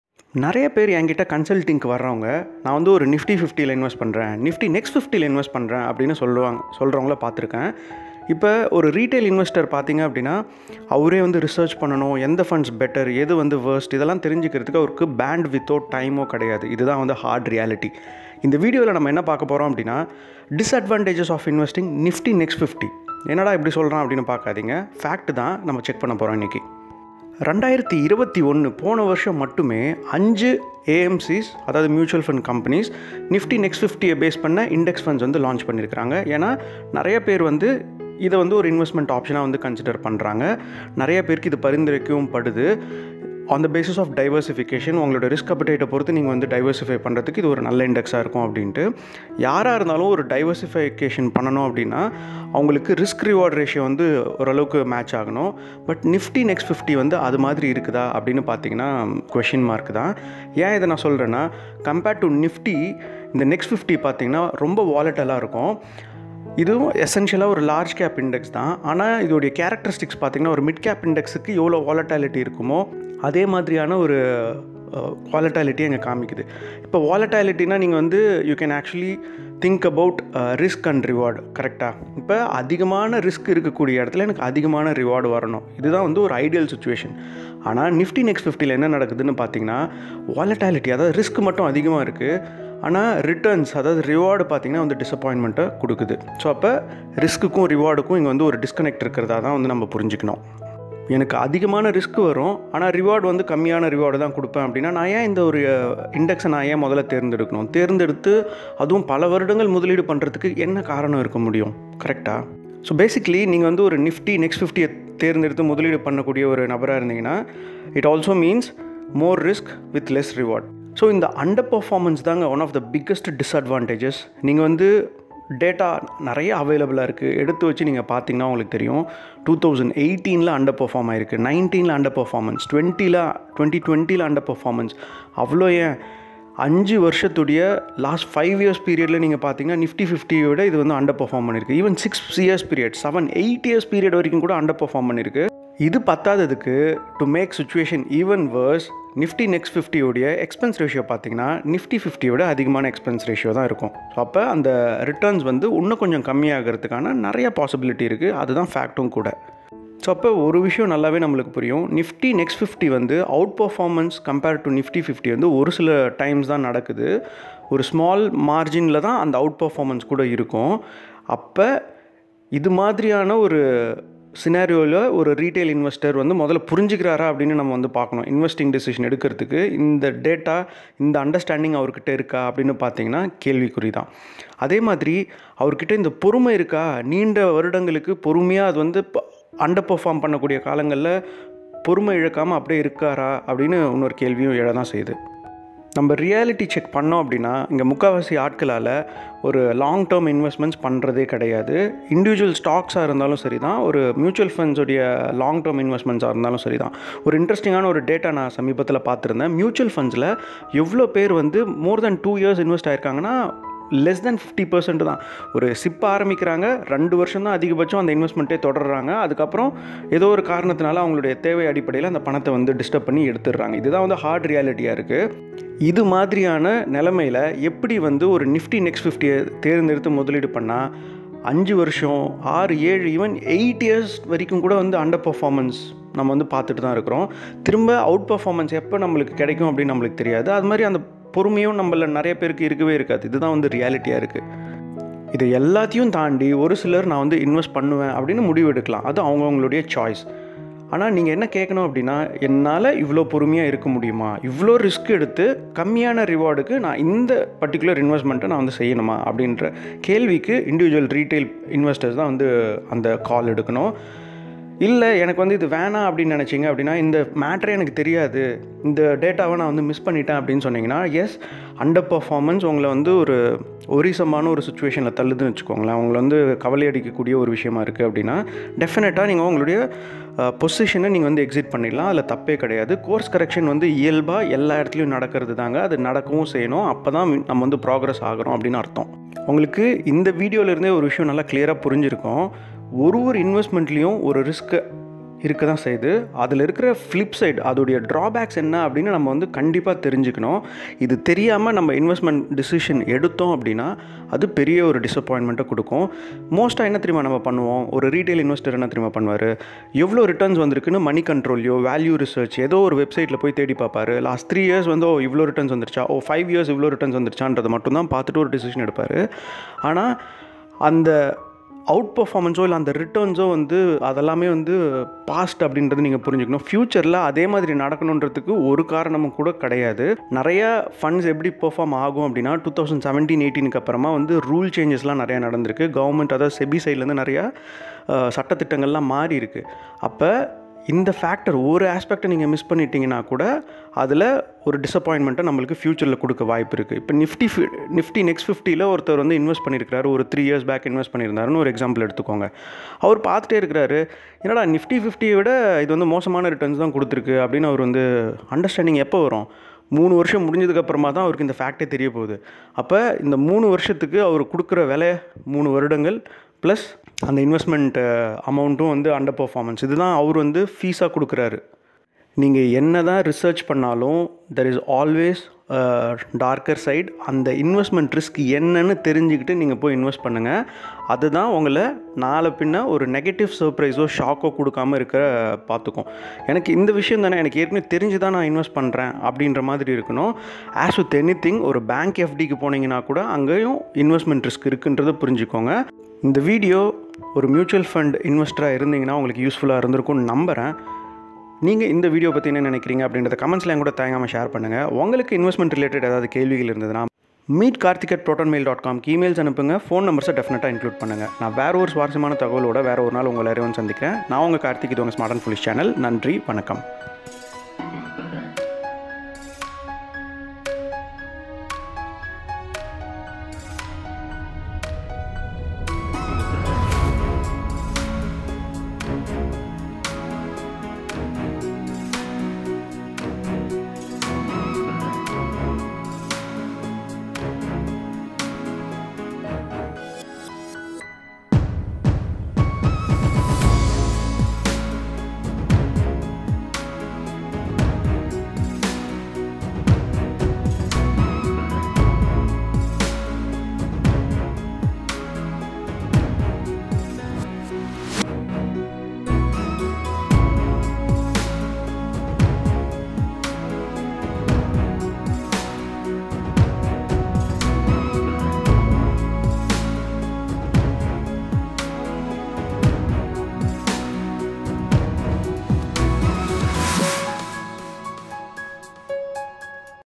Disadvantages of Nifty Next 50 | Nifty Next 50 Index Fund Tamil
Risks of investing in Nifty Next50